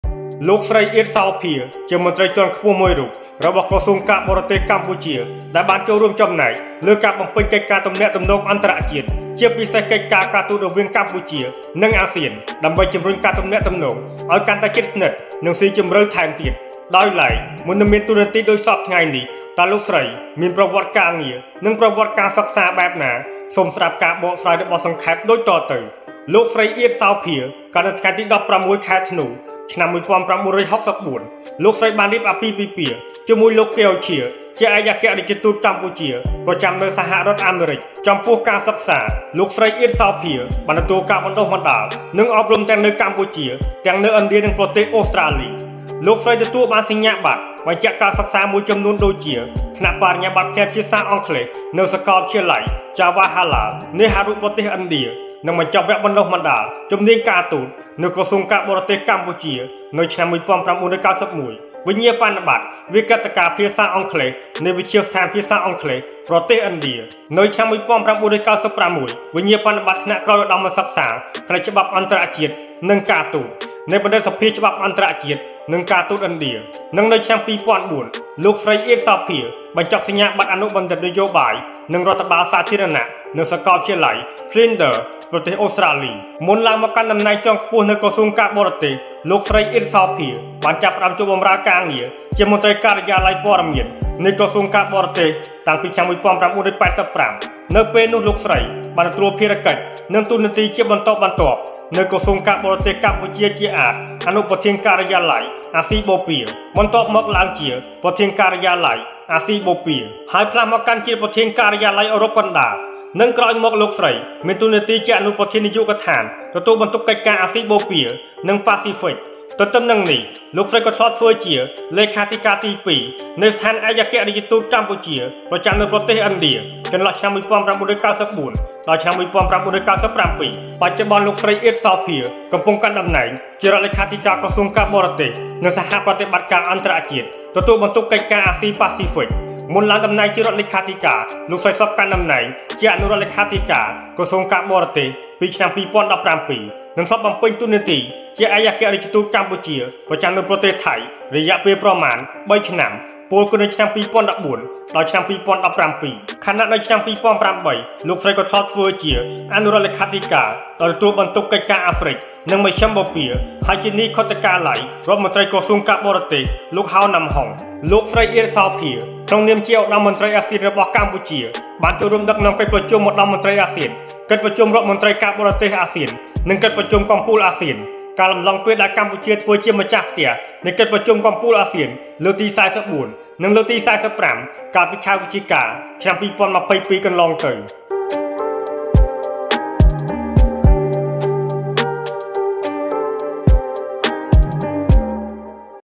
ប្រវត្តិ សង្ខេប លោកស្រី អ៊ាត សោភា-History of Mrs. Eat Sophea by សង្ខេប Sangkeb
លោកស្រី អ៊ាត សោភា
Mrs. Eat Sophea
ជារដ្ឋលេខាធិការក្រសួងការបរទេស

Post by សង្ខេប sangkeb
​​video+photo by fb  ជារដ្ឋលេខាធិការក្រសួងការបរទេស
via : kampucheathmey